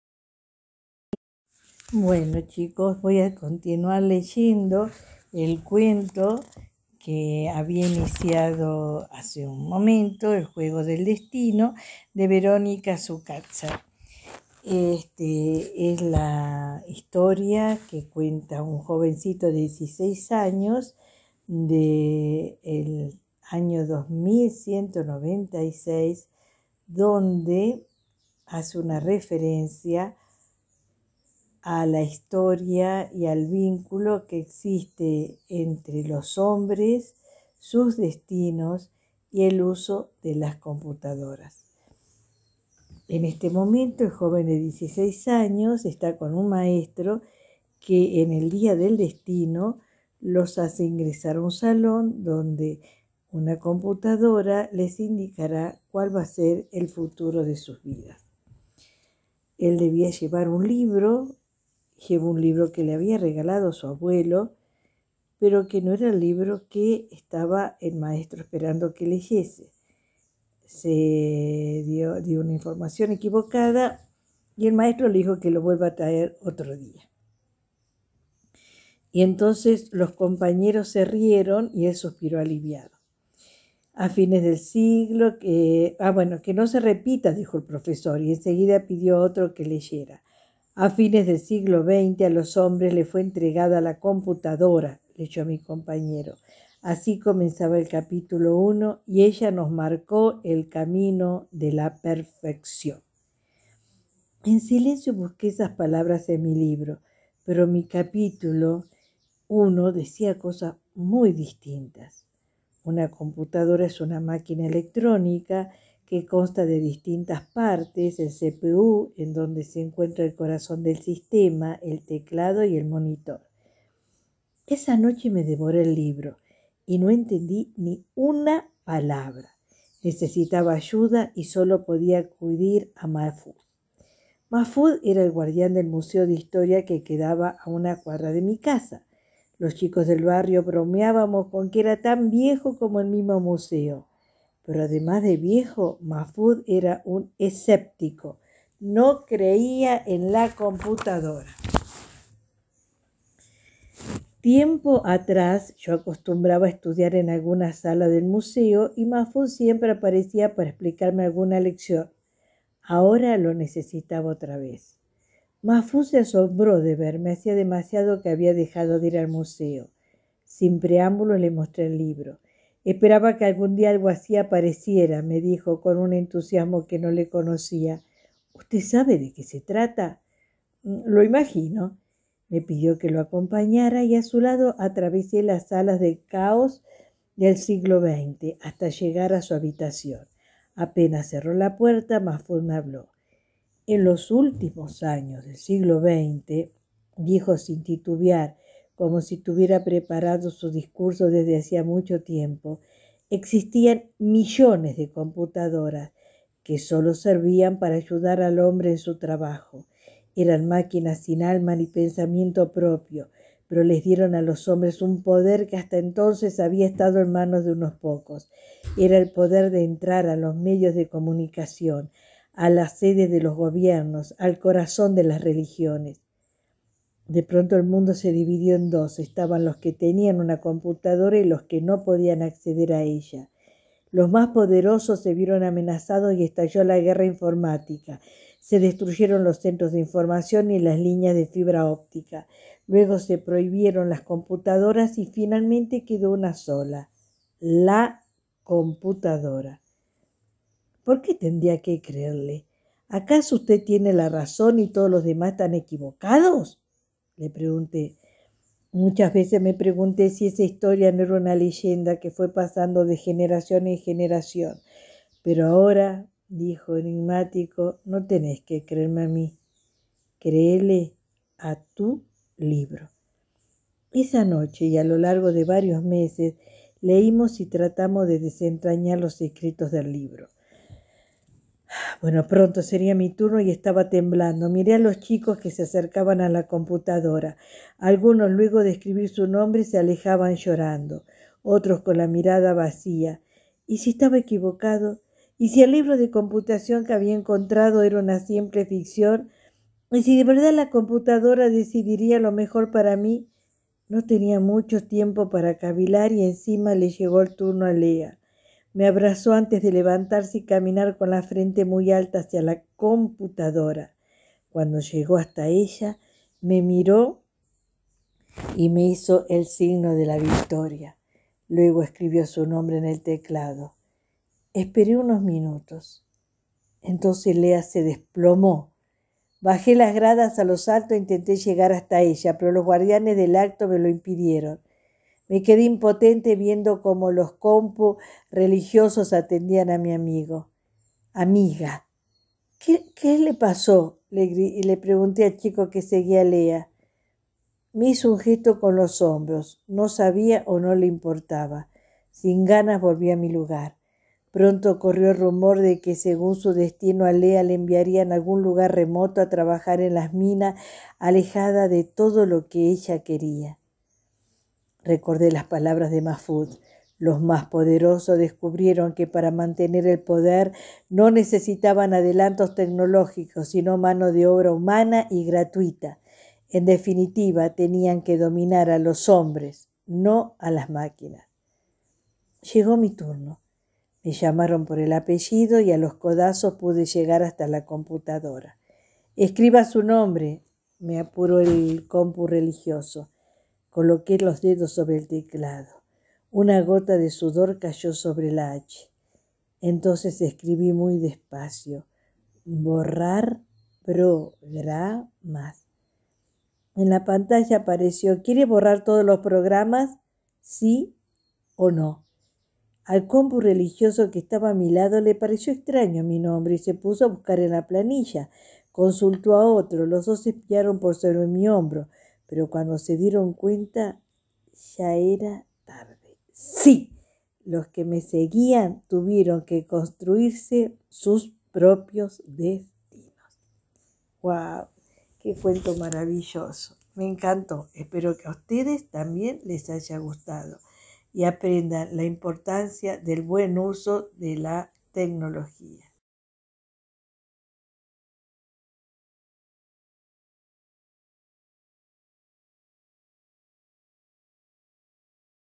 La abuela Gladis nos lee "El juego del destino", de Verónica Sukaczer (Parte 2).
Verónica Sukaczer (Buenos Aires, 1968) es una periodista, escritora y editora argentina.
"El juego del destino" se incluye en su libro "Nunca confíes en una computadora", publicado en 2007.